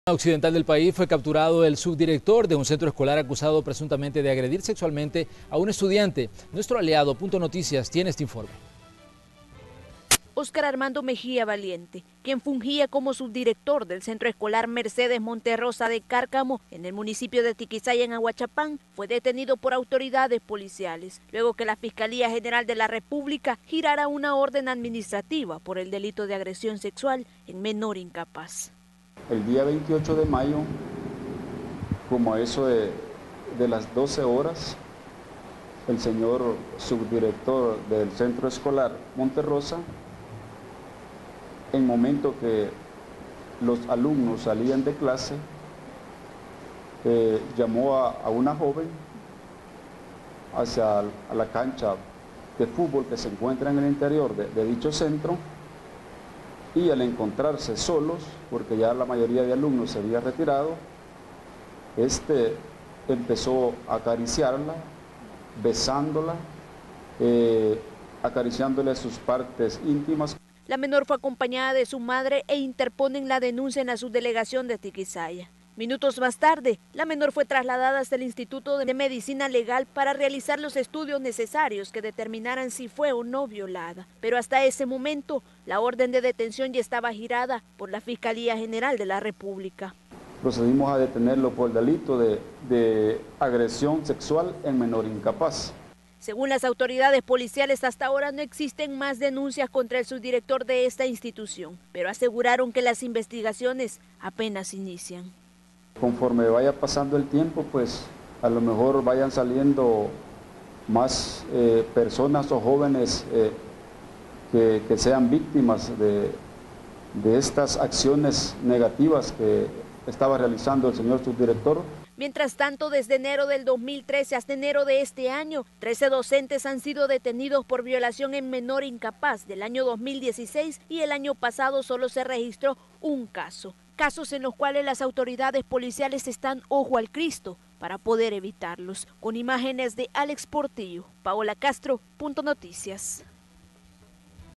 Detienen a subdirector educativo por agresión sexual
Este miércoles fue capturado el sub director de un centro escolar acusado de agredir sexualmente a un estudiante.